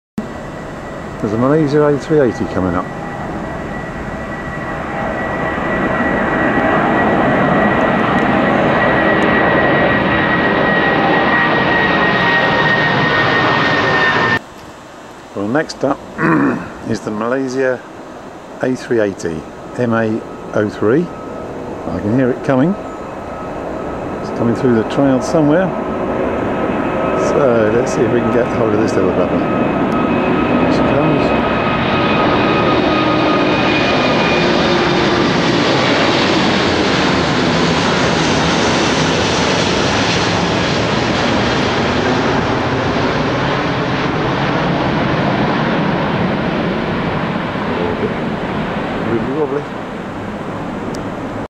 A380 Malaysia Airlines take off over the neighbourhood - Heathrow
I got Flight Radar wrong - so false alarm - 747 BA followed by Malaysia - it has only been out of Heathrow for a few weeks and makes for a great sight!